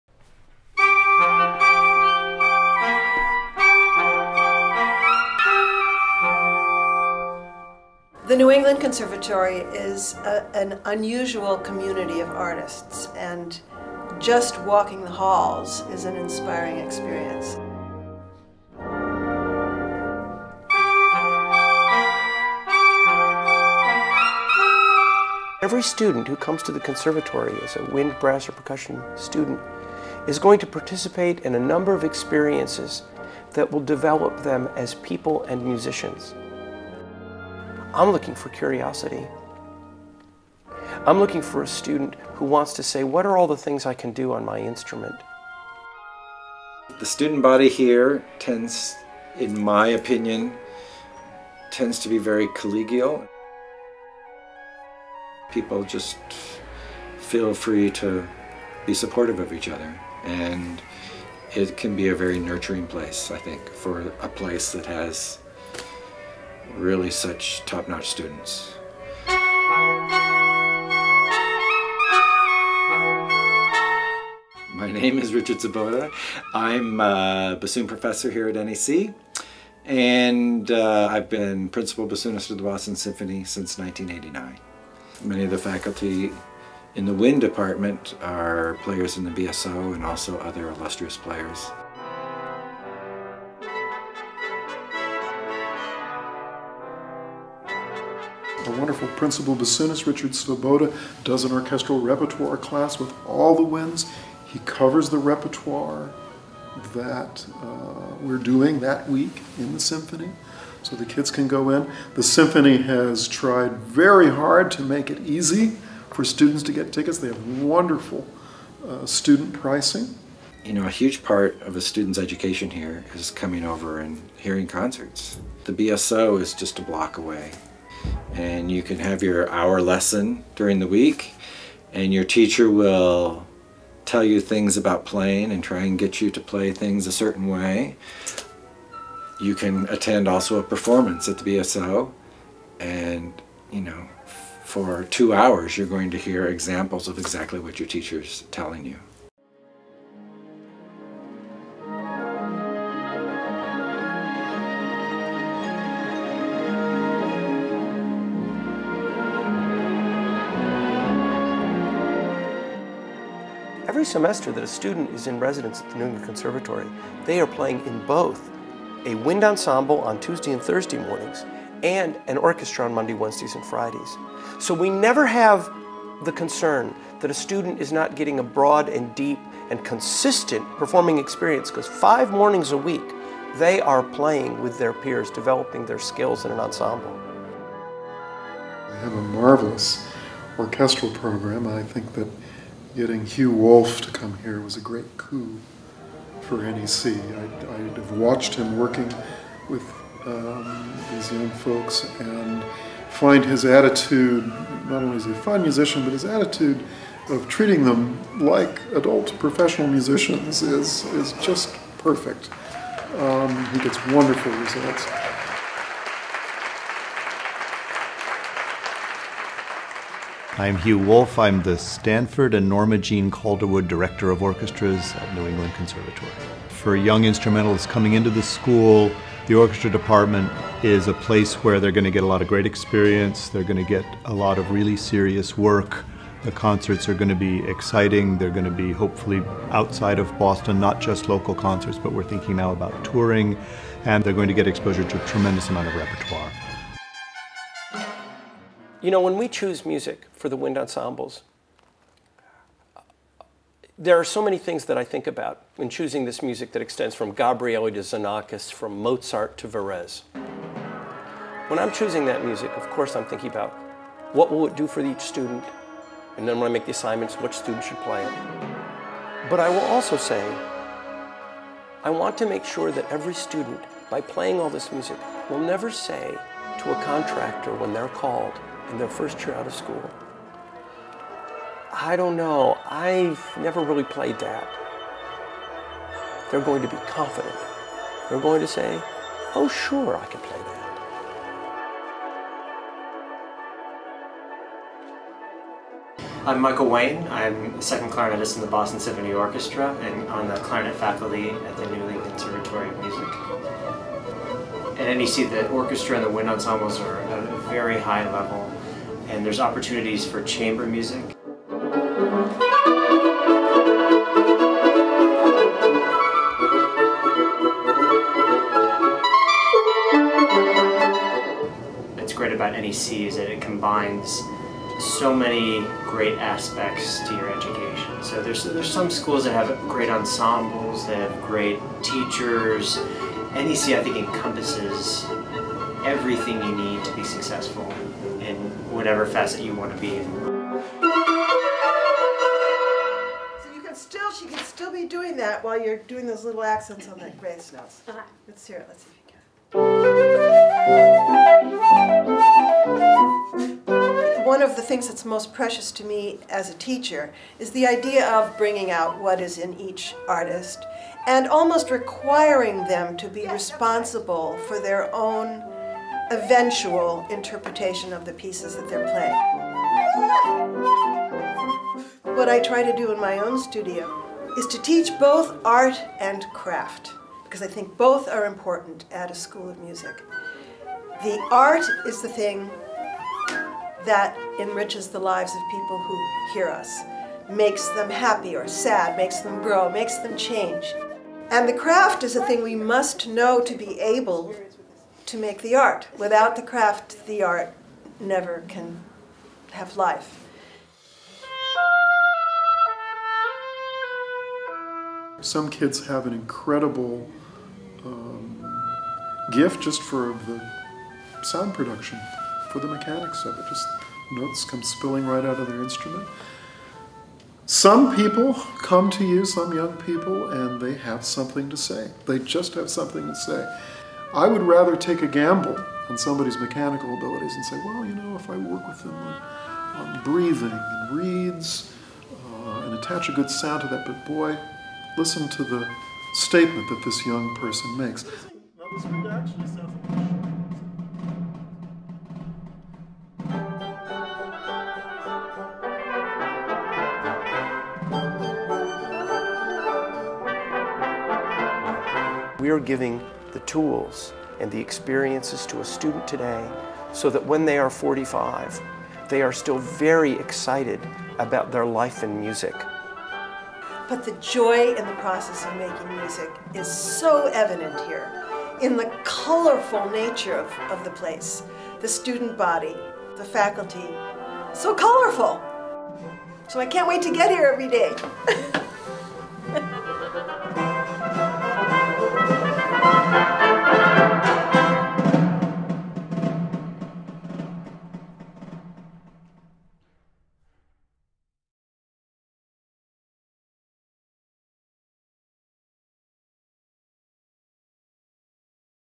NEC Woodwind Studies Department
Under the direction of chair Richard Svoboda, New England Conservatory's Woodwind Studies Department offers one of the finest music educations available. In this video you can meet several of our outstanding faculty members, find out about NEC's close relationship to the Boston Symphony, and hear about the great performance and instruction opportunities enjoyed by our students.